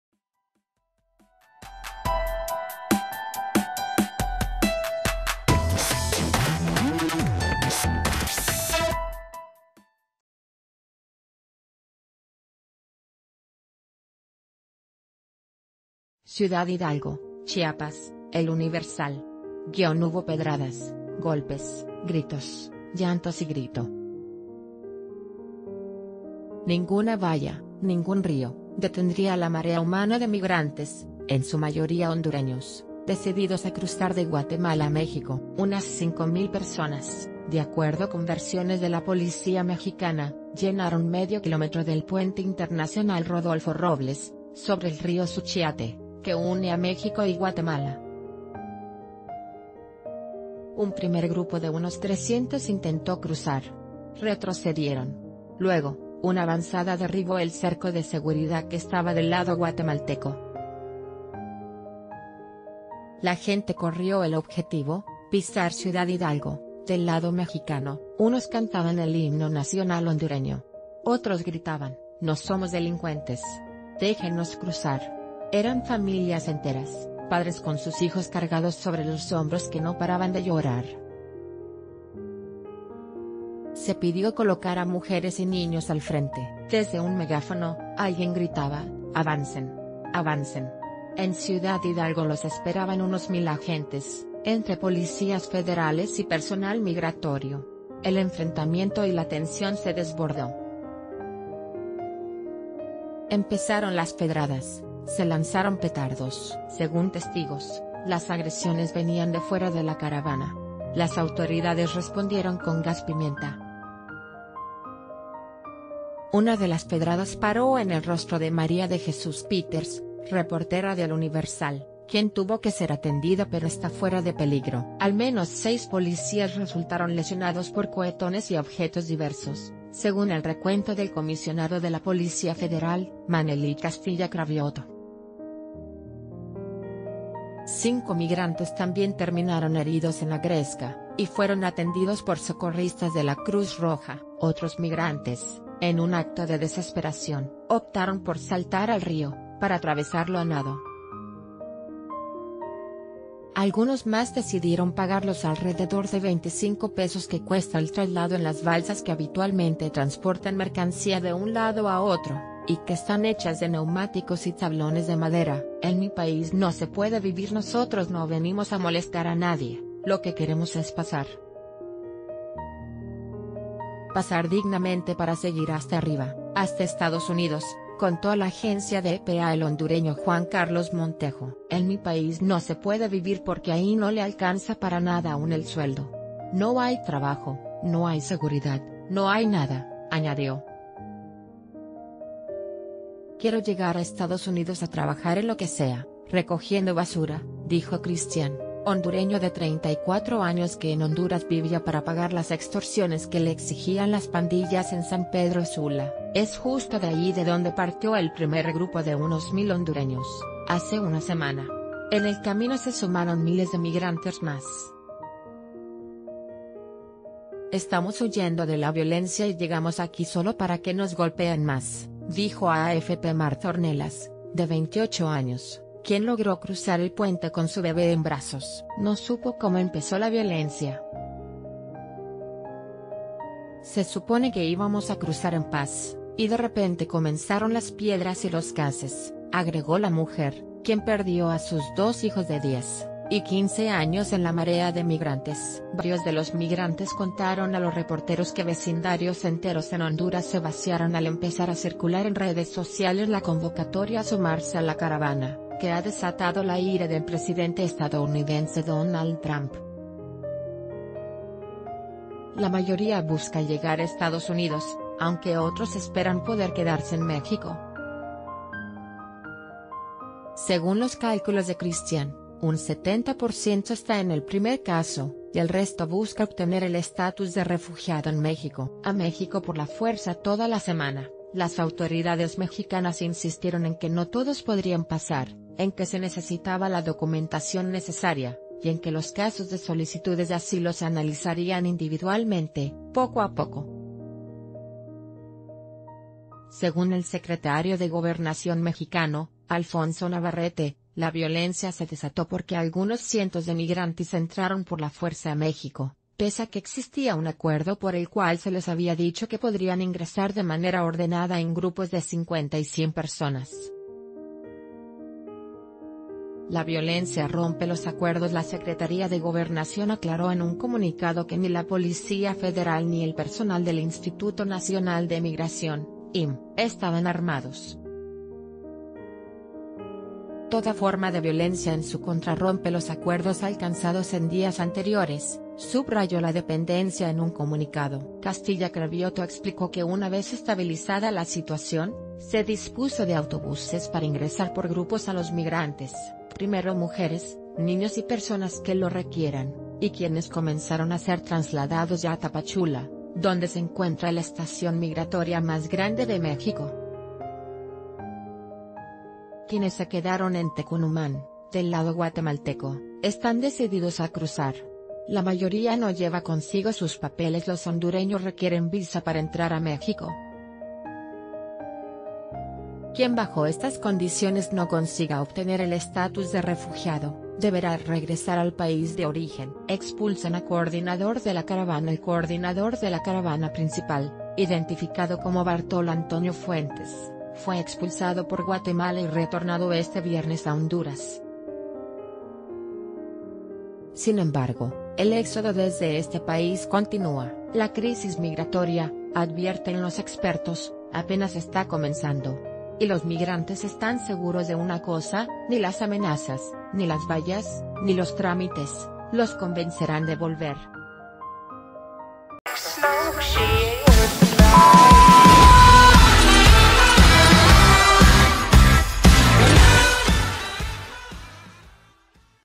Migrantes hondureños gritan: “No somos delincuentes” - El Diario de Yucatán
CIUDAD HIDALGO, Chiapas (El Universal).- Hubo pedradas, golpes, gritos, llantos y grito. Ninguna valla, ningún río, detendría a la marea humana de migrantes, en su mayoría hondureños, decididos a cruzar de Guatemala a México. Unas cinco mil personas, de acuerdo con versiones de la policía mexicana, ...